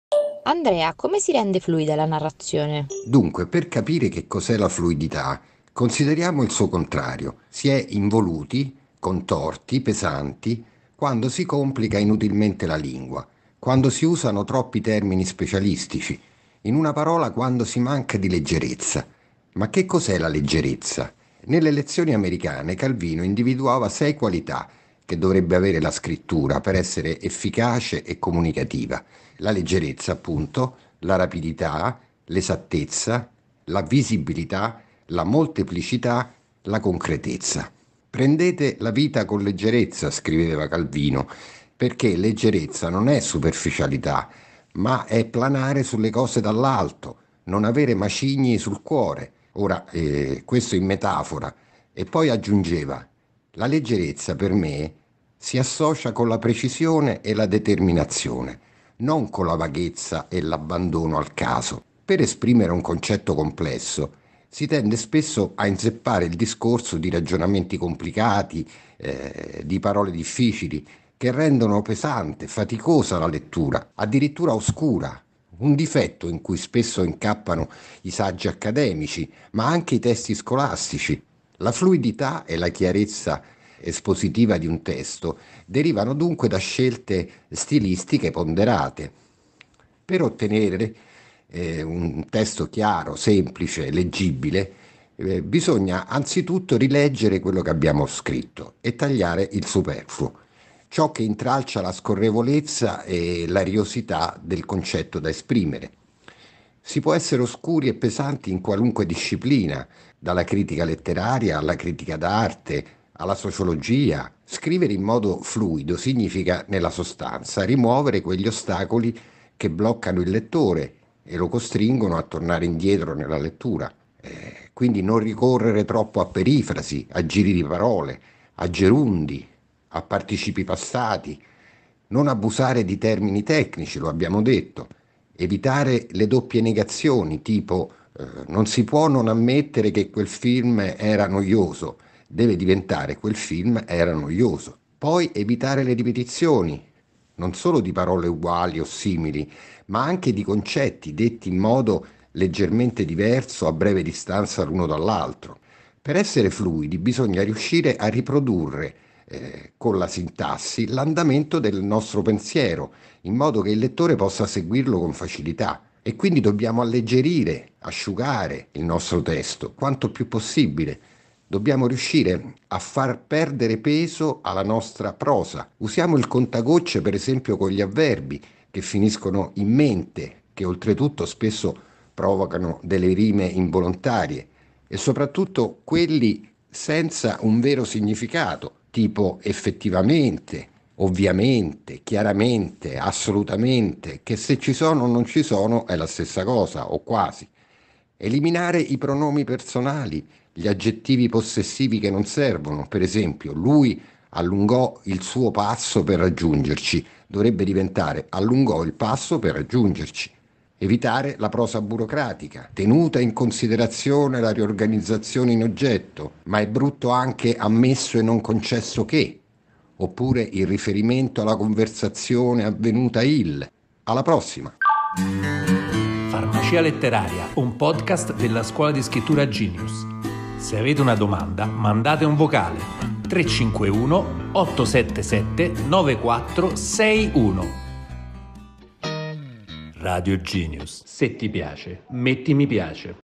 410 – RENDERE FLUIDA LA NARRAZIONE [scrittura creativa]
Nelle lezioni americane Calvino individuava sei qualità che dovrebbe avere la scrittura per essere efficace e comunicativa: la leggerezza, la rapidità, l’esattezza, la visibilità, la molteplicità e la concretezza.
Risponde Andrea Carraro, scrittore, ha pubblicato romanzi e racconti fra cui “Il branco” (Theoria, 1994), diventato un film di Marco Risi (sceneggiato a quattro mani dallo scrittore e dal regista). Ha pubblicato anche “L’erba cattiva” (Giunti, 1996), “La ragione del più forte” (Feltrinelli, 1999),  “Non c’è più tempo” (Rizzoli, 2002) (Premio Mondello), “Tutti i racconti” (Melville, 2013),  “Sacrificio” (Castelvecchi, 2017).